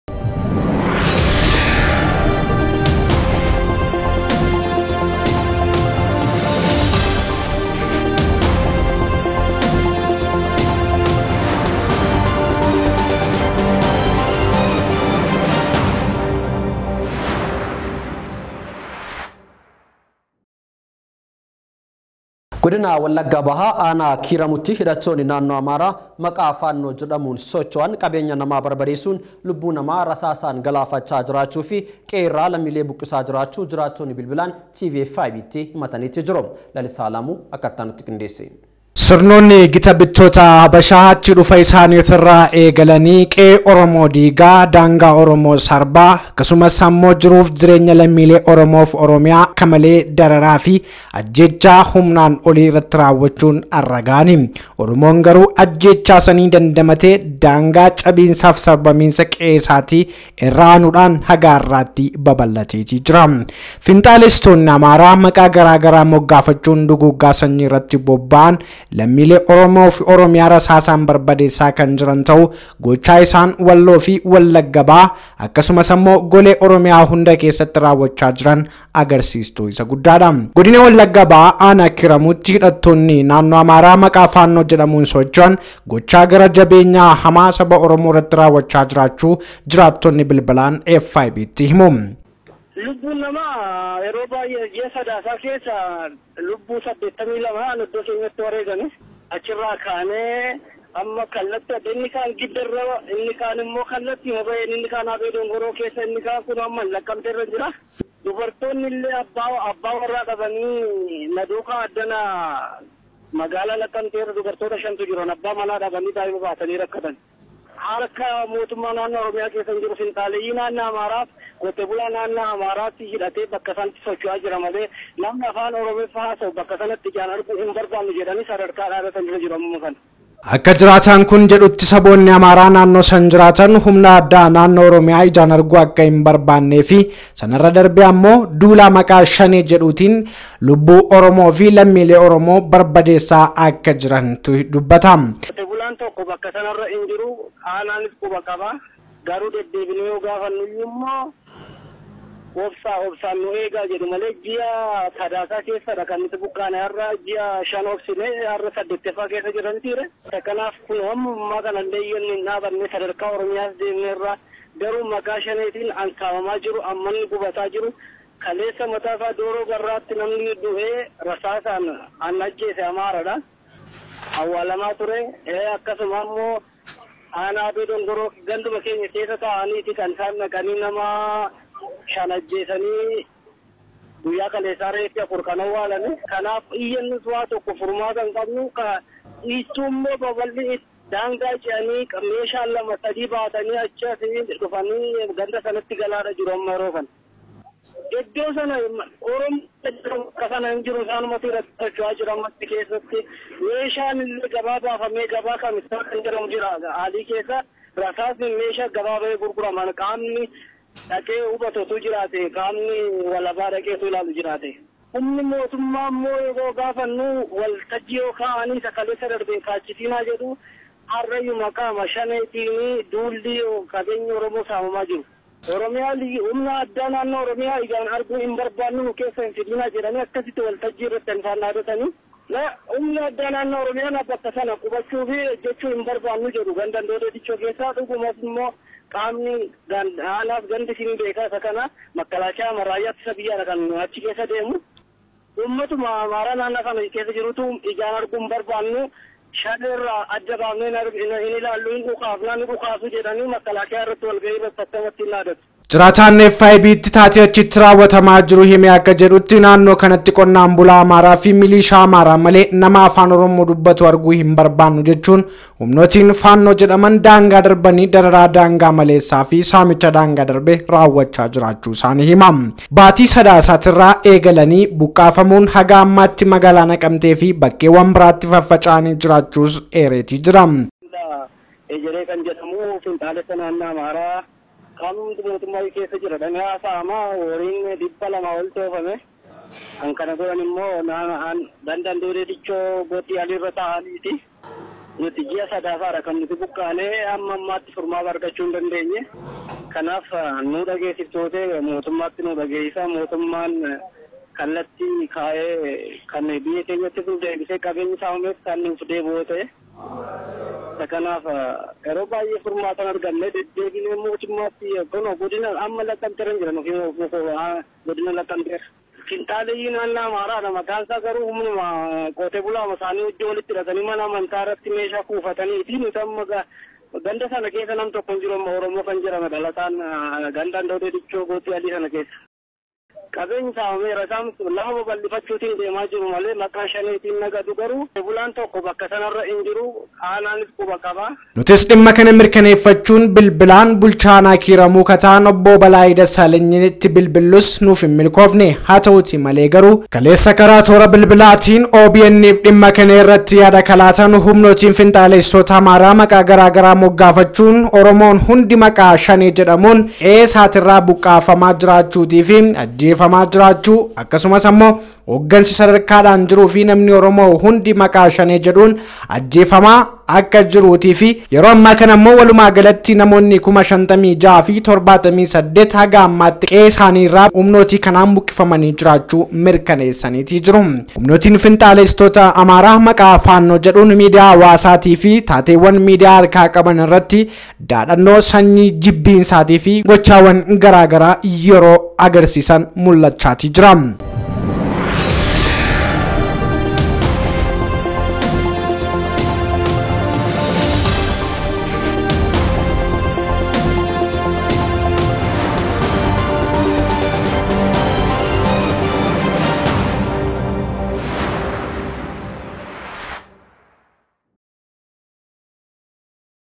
W/B Aanaa Kiiramuutti Humnootiin Finxaalessitoota Amaaraa Saamichaafi haleellaa raawwachuun himame.
FiB:  W/B Aanaa Kiiramuutti Humnootiin Finxaalessitoota Amaaraa Saamichaafi haleellaa raawwachuu jiraattonni himatan.(April.20,2022) Finfinnee